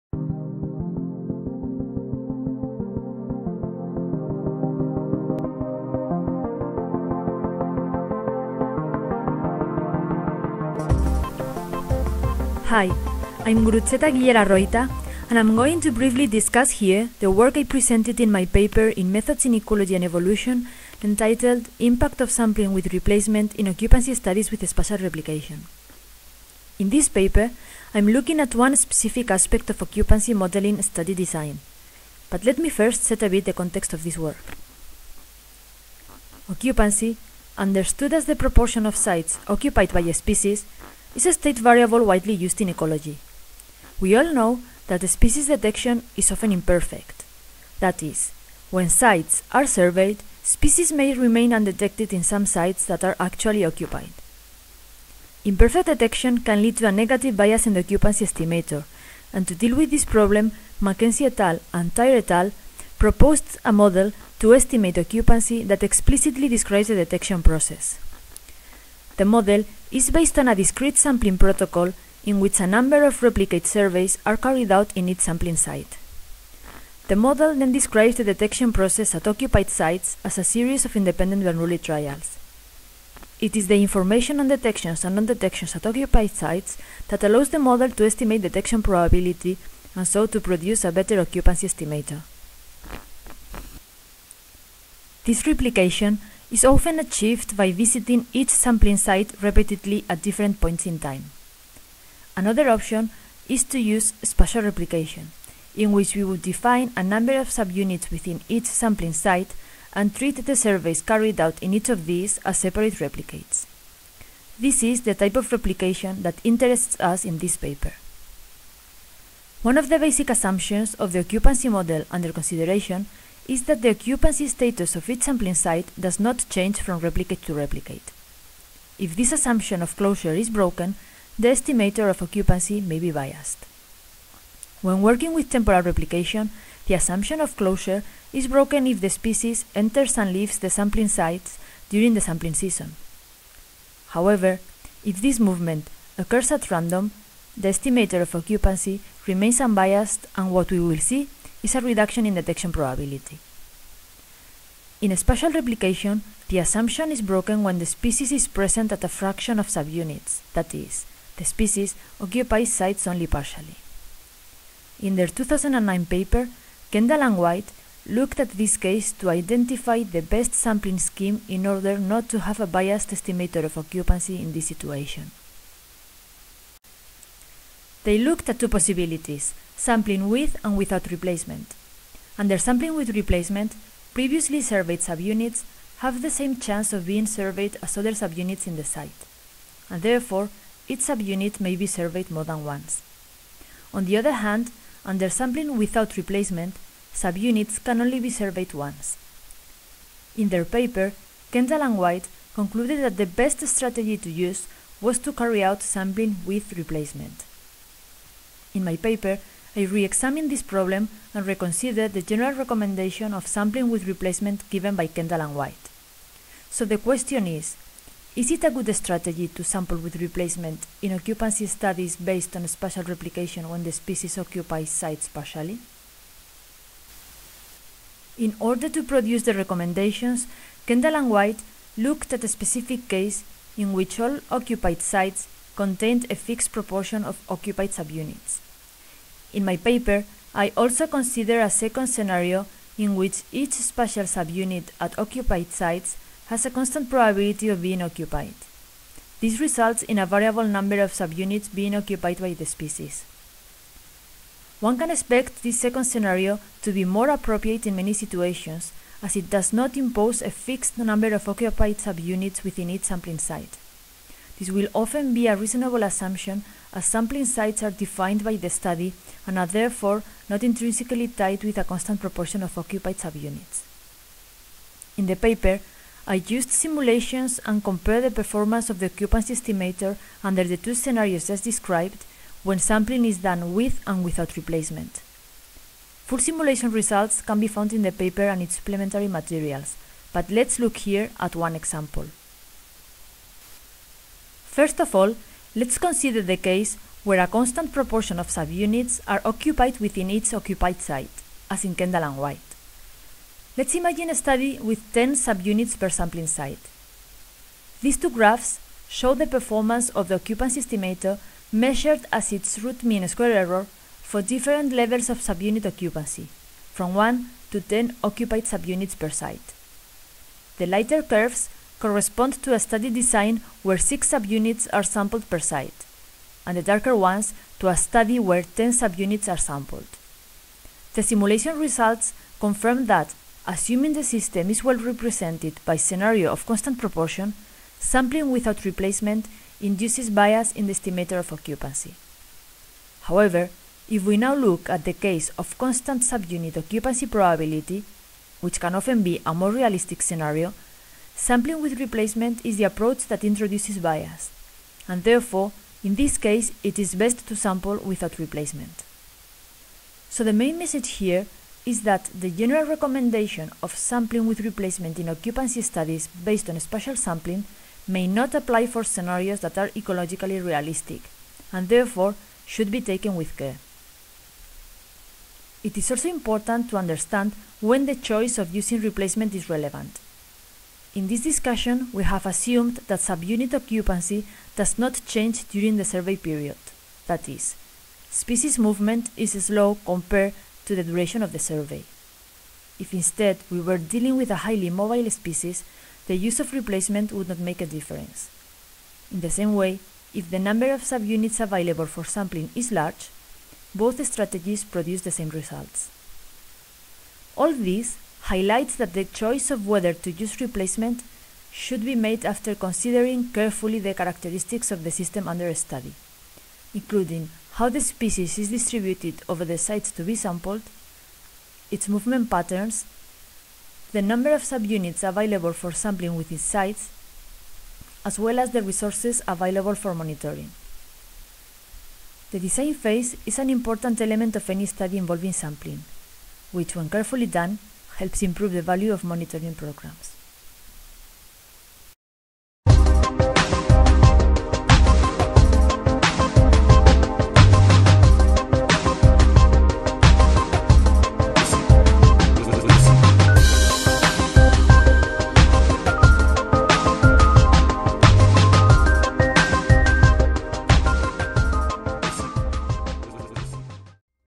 The impact of sampling with replacement
Gurutzeta Guillera-Arroita explains the ideas behind their latest paper, published in Methods in Ecology and Evolution, looking at the impact of sampling with replacement in ecological studies of species occupancy.

Gurutzeta discusses the impact of partial occupancy on such studies, highlighting the necessity of a careful understanding of a system before deciding whether to sample with replacement.